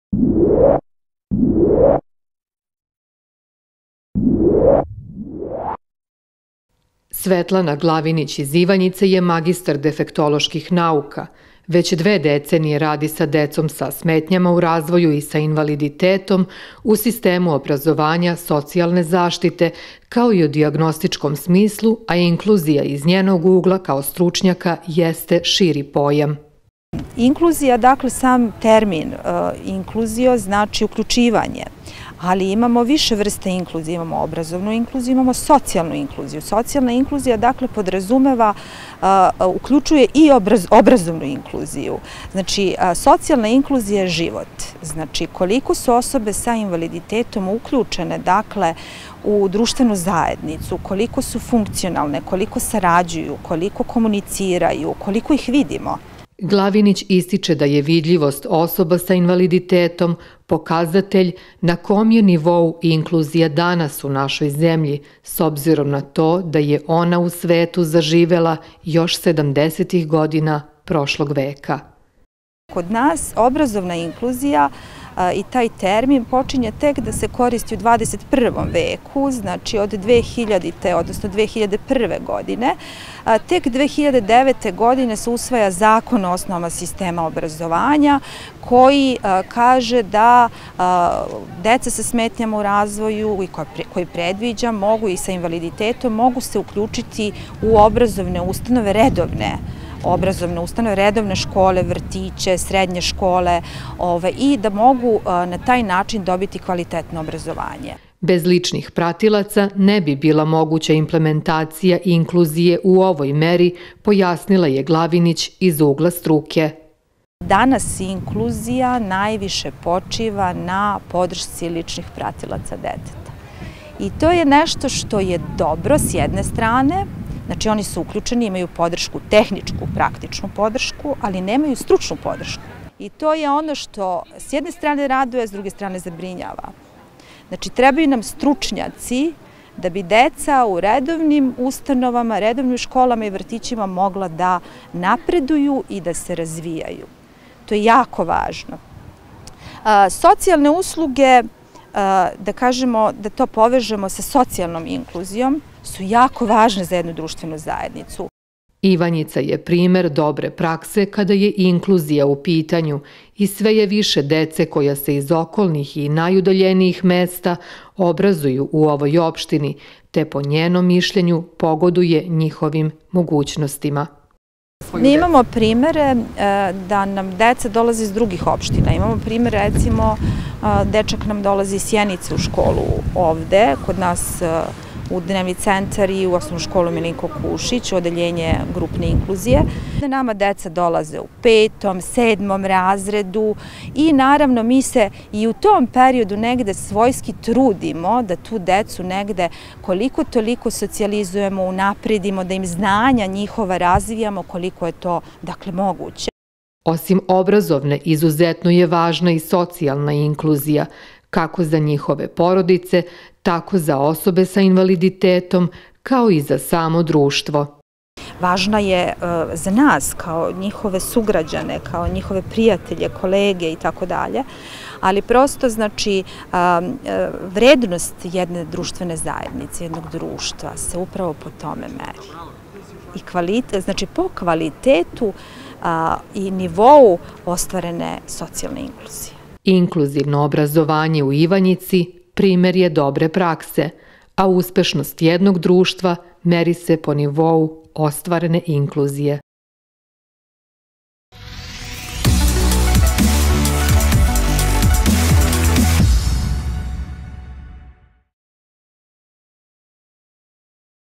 Inkluzija u Ivanjici je značajan faktor po čemu se i meri uspešnost jedne društvene zajednice
Portal za osobe sa invaliditetom zapadne Srbije